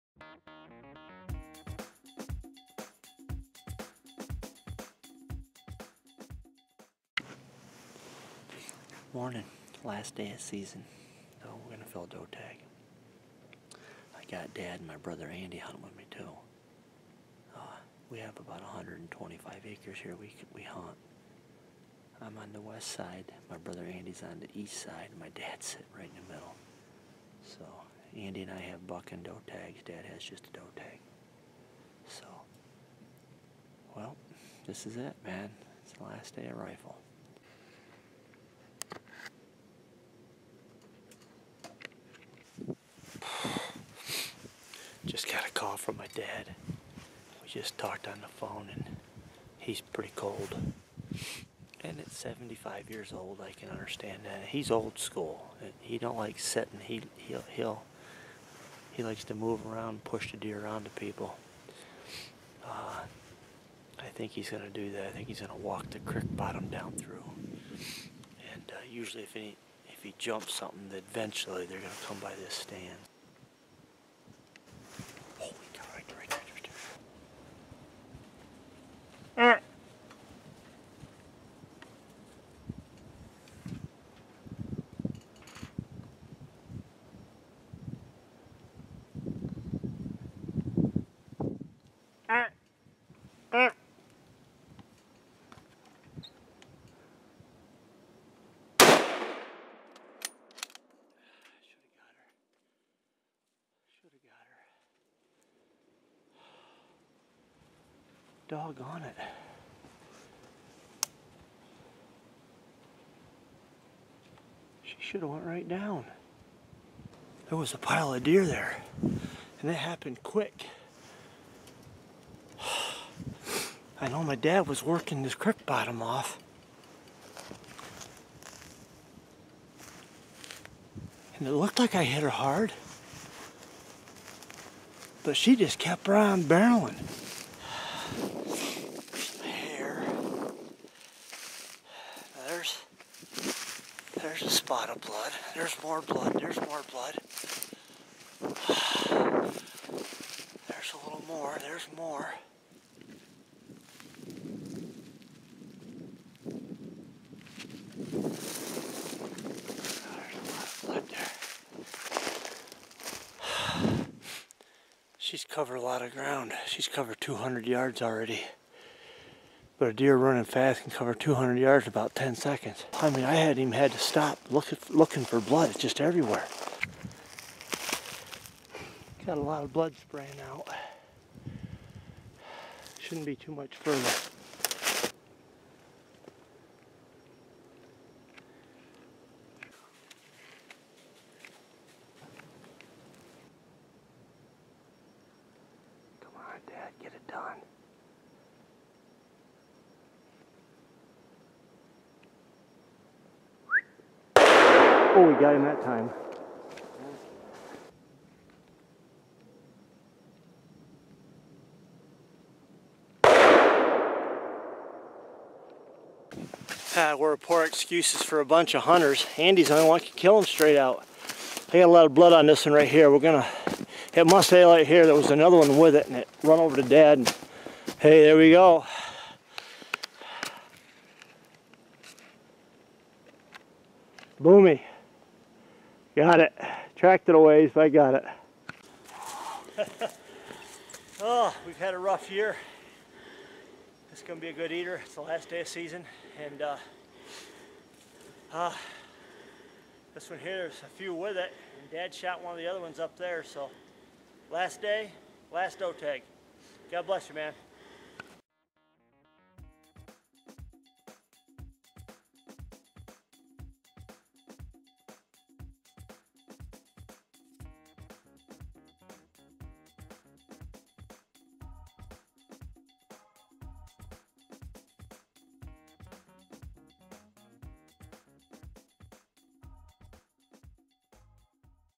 Episode # 30 Last day deer double
Join Tom of HOWE2OUTDOORS as he shoots a doe on the last day of season and gets a bonus hunt of his Dad shooting another doe from a distance.